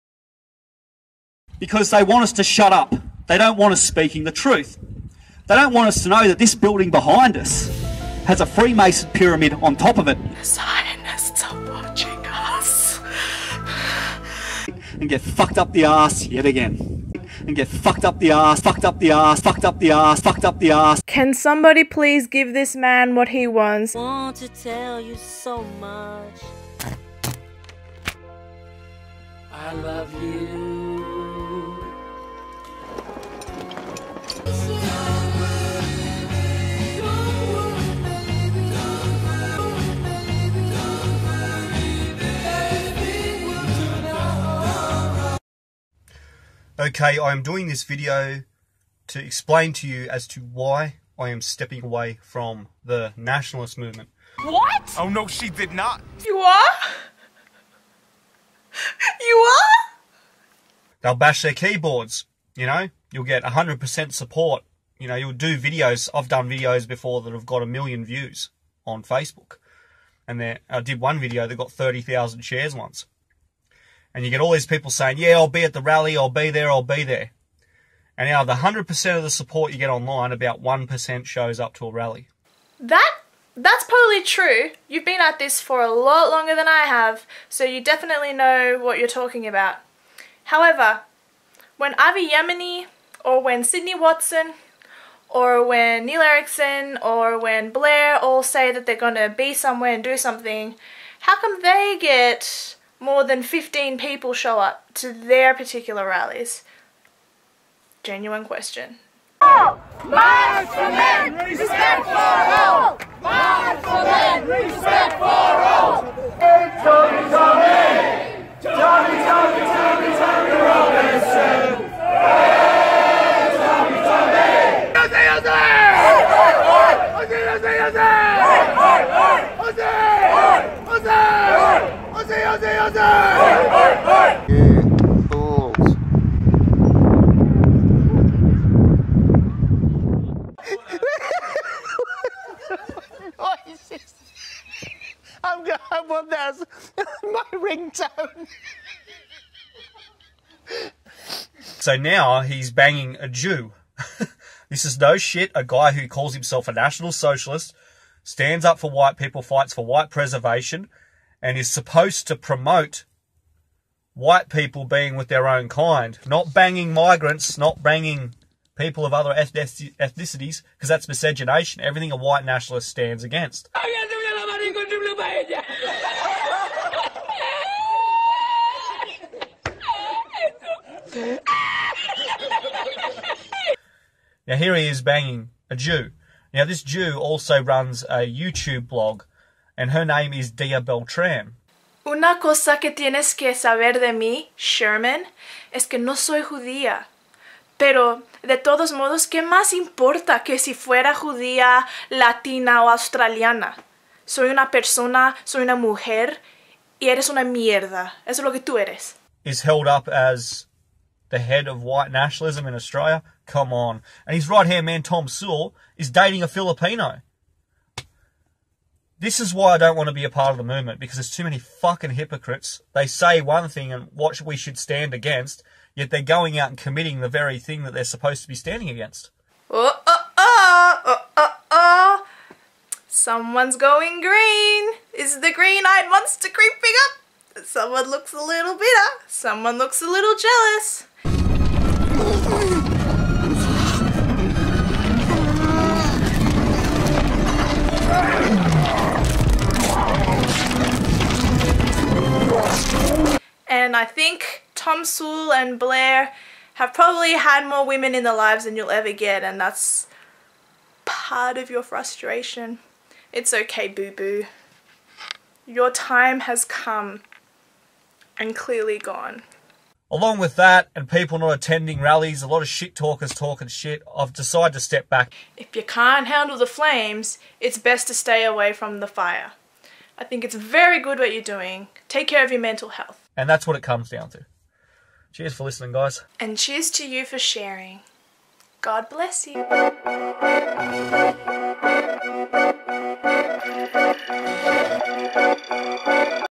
Poor Shermon Burgess
Dia Beltran: In this video I respond to Shermon Burgess.